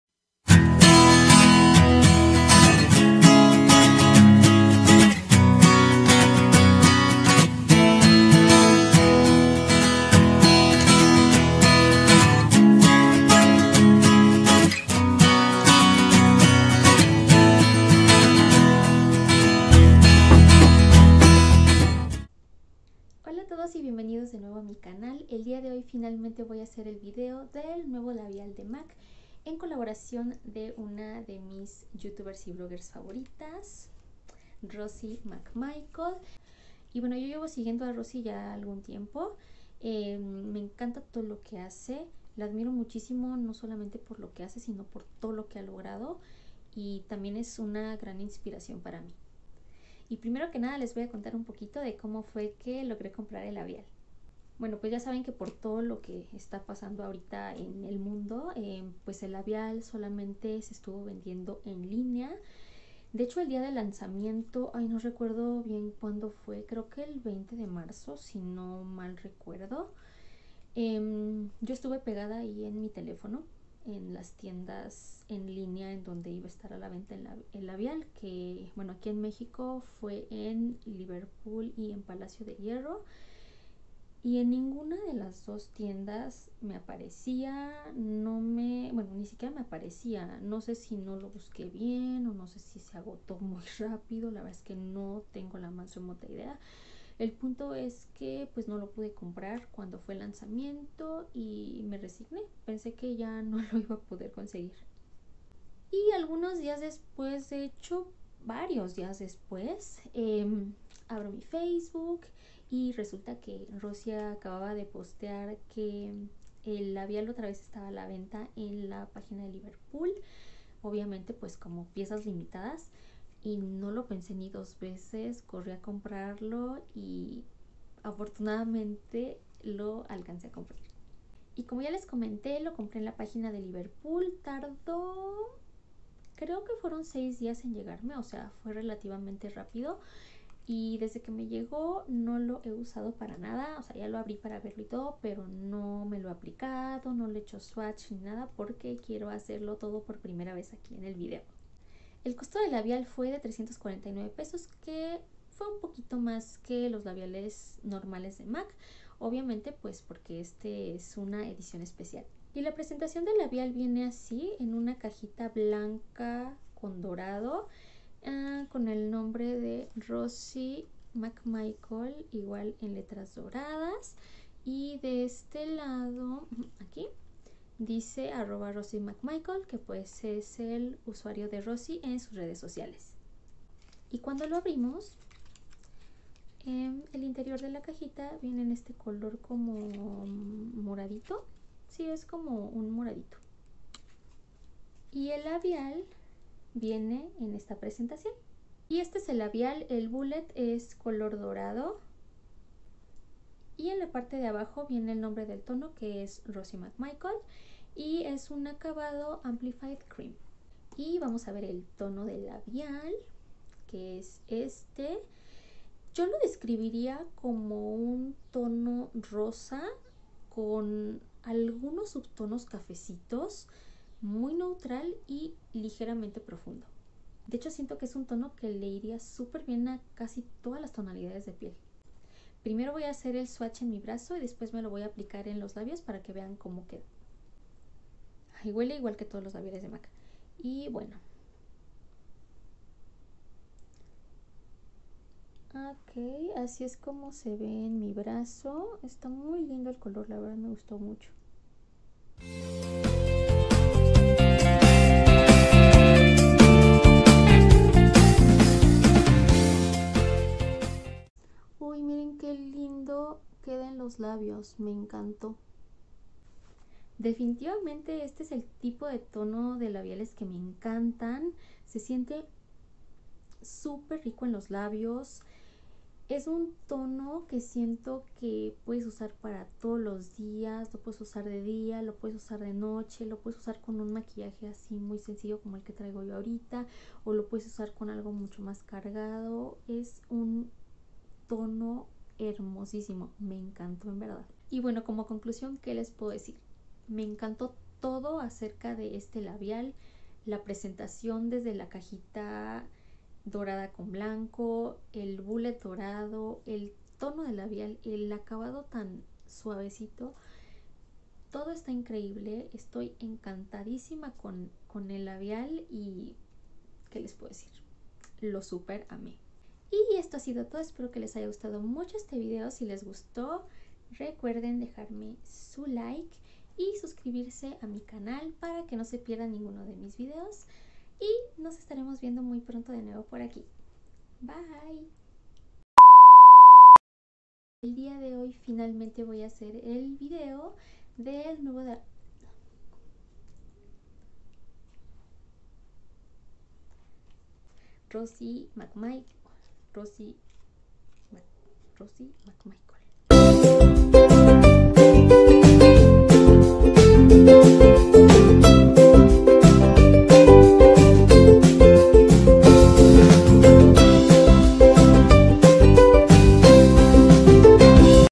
Reseña del labial MAC Maker X Rosy McMichael ¿Me Gustó?
Por fin pude comprar mi tan esperado labial de MAC en colaboración con Rosy McMichael. Aquí les dejo la reseña y mis primeras impresiones, espero que les guste y les sea útil el vídeo.

URL: InShot

URL: InShot